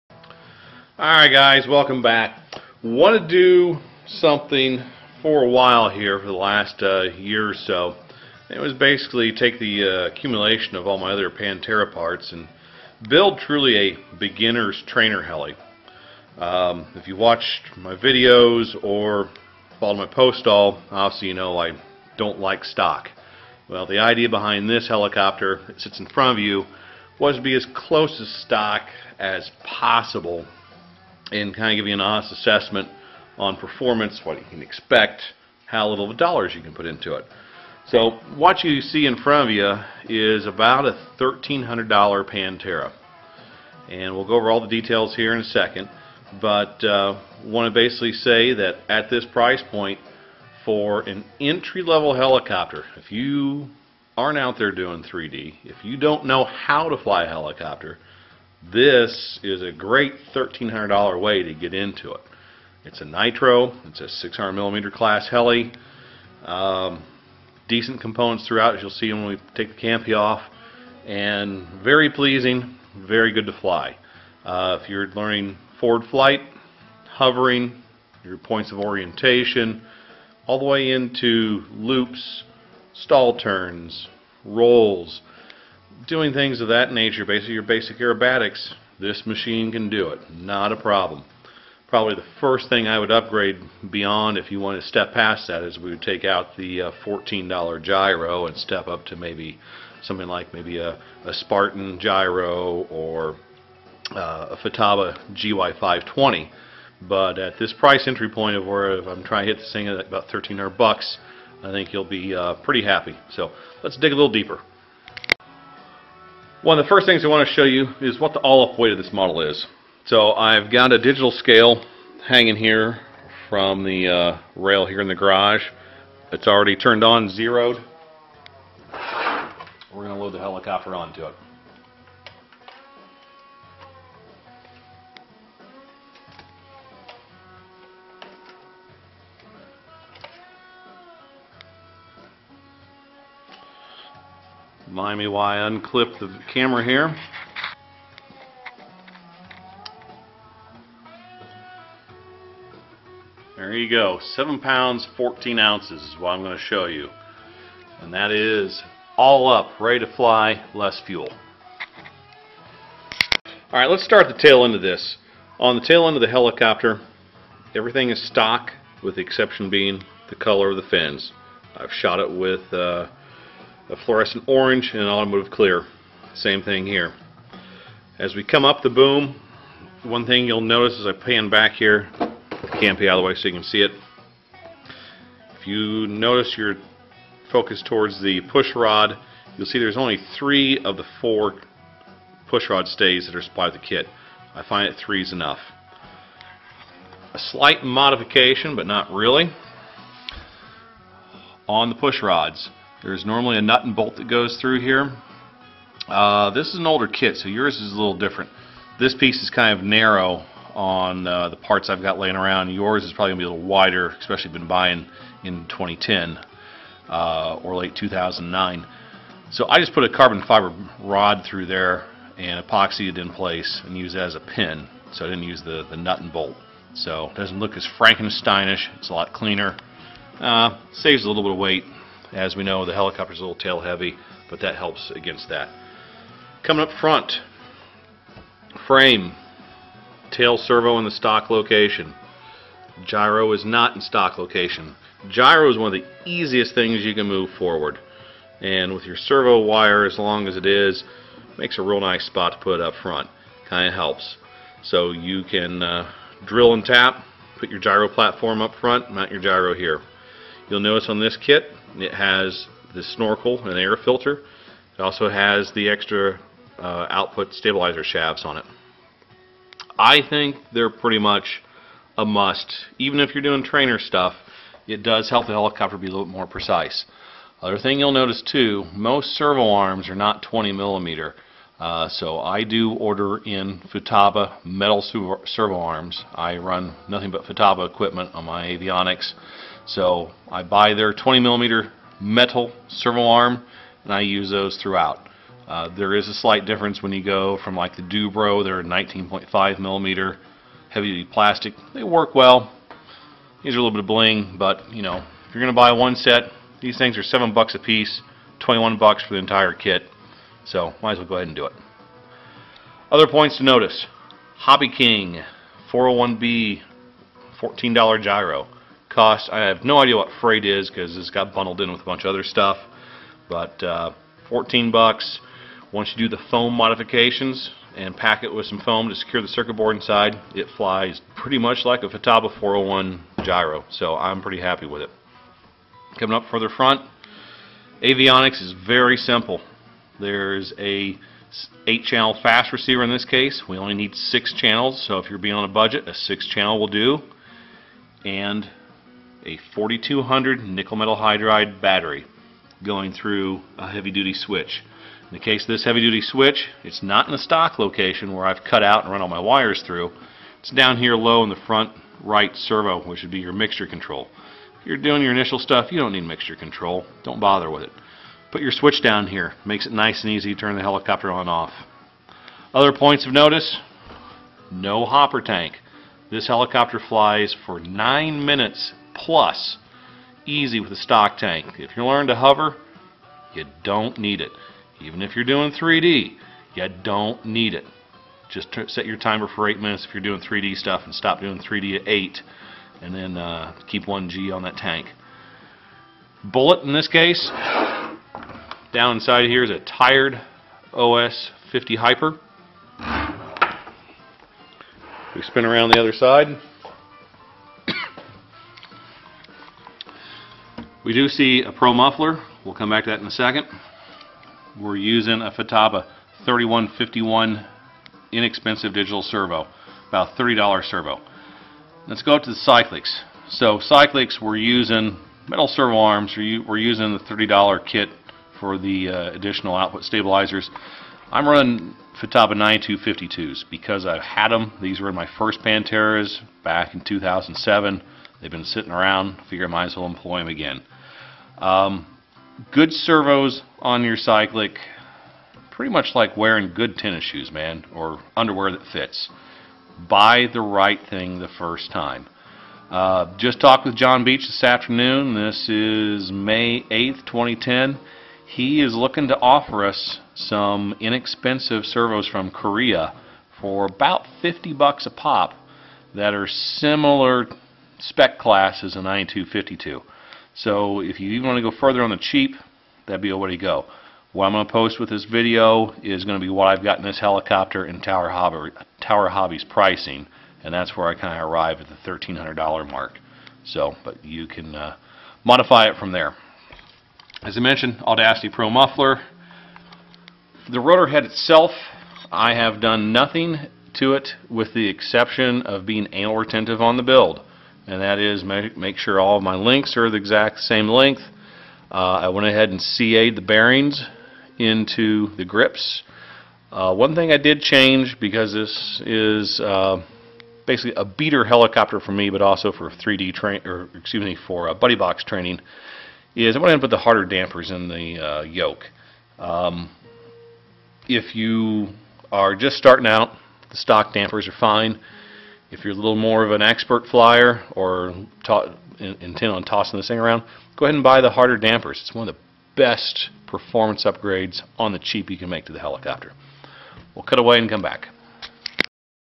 Pantera on the cheap Part 1.mp4
Building a $1300.00 Nitro, 600mm Class Helicopter for training, Basic Aerobatics & mild 3D Flight. Part 1